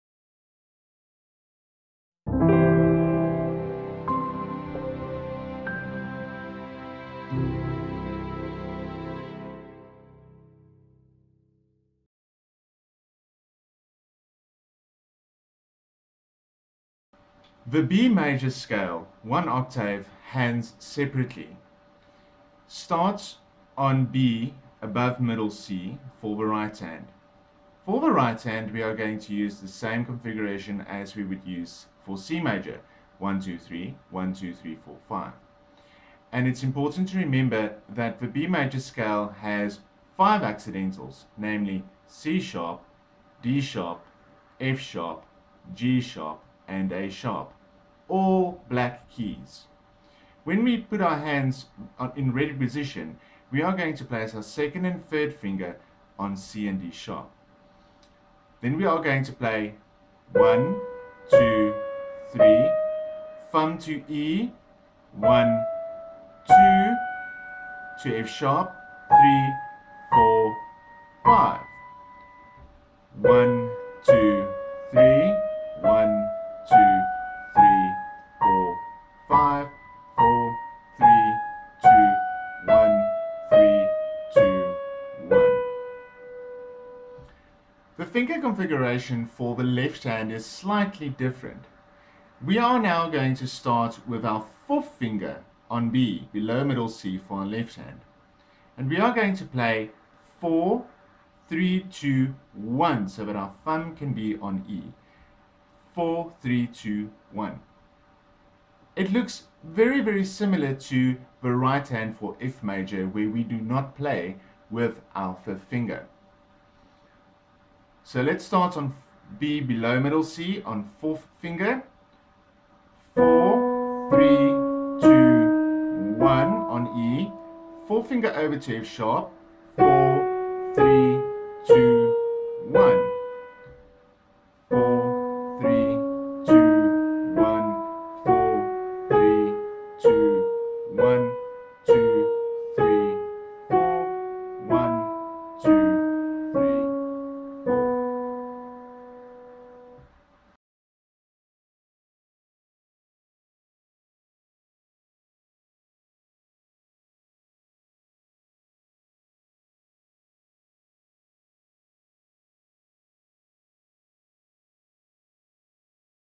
Piano Scales - B Major, 1 Octave, Hands Separately
In this scale video, I demonstrate how to play the B Major Scale for one octave, hands separately. I also explain what fingering to use as well as what accidentals to play.